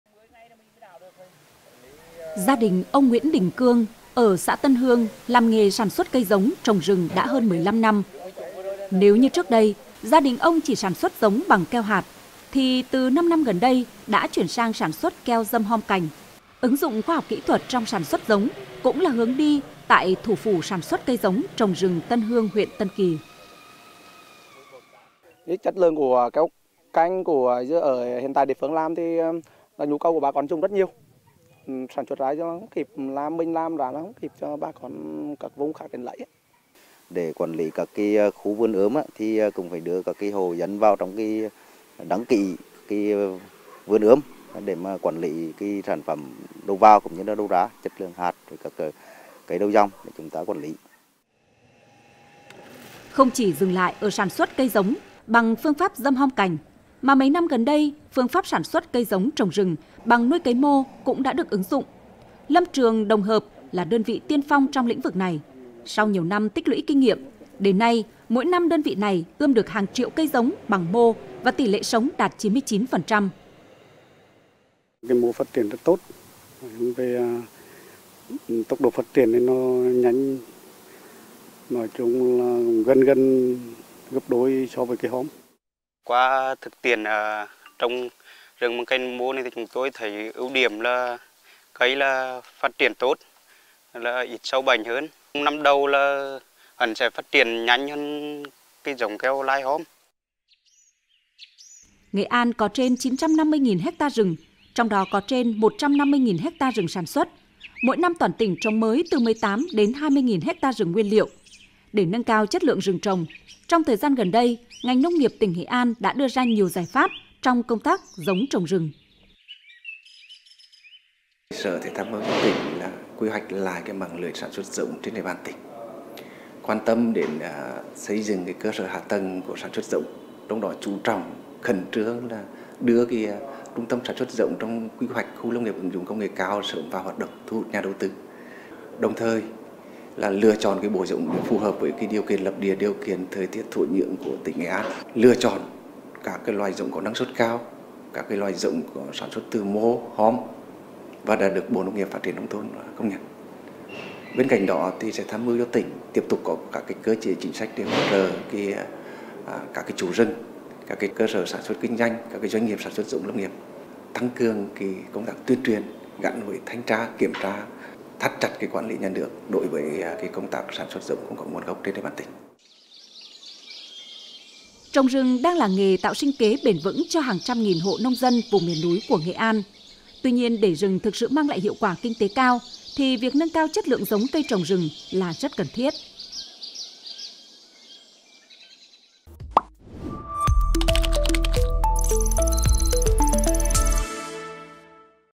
Nâng cao chất lượng rừng từ cây giống
Với gần 1 triệu ha rừng, Nghệ An là tỉnh có diện tích rừng lớn nhất cả nước, trong đó diện tích rừng trồng khoảng 200 nghìn ha Diện tích rừng trồng lớn nên nhu cầu cây giống trồng rừng của Nghệ An cũng rất cao. Trong xu thế hiện nay, trồng rừng gỗ lớn đang là hướng đi mang lại giá trị kinh tế cao. Để làm được điều này việc nâng cao chất lượng, nguồn gốc cây giống trồng rừng là rất cần thiết.